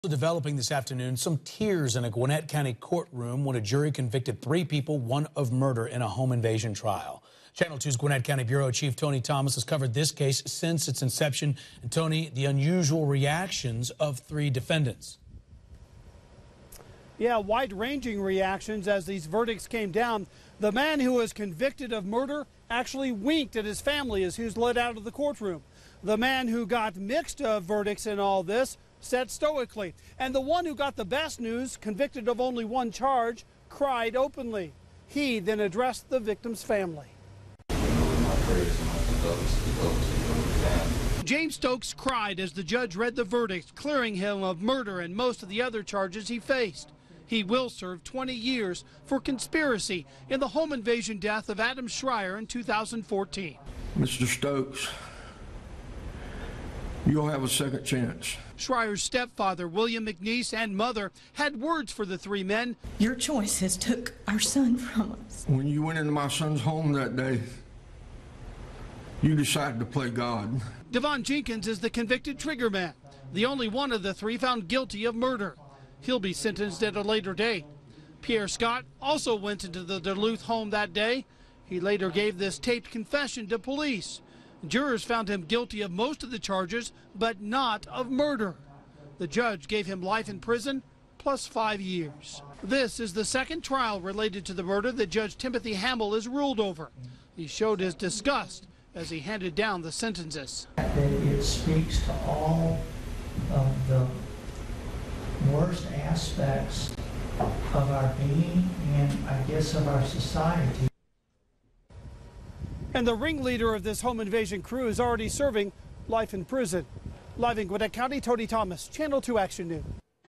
Jury finds 1 guilty, 2 not guilty in deadly home invasion
There were tears in the Gwinnett County courtroom when the jury convicted three people in a home invasion.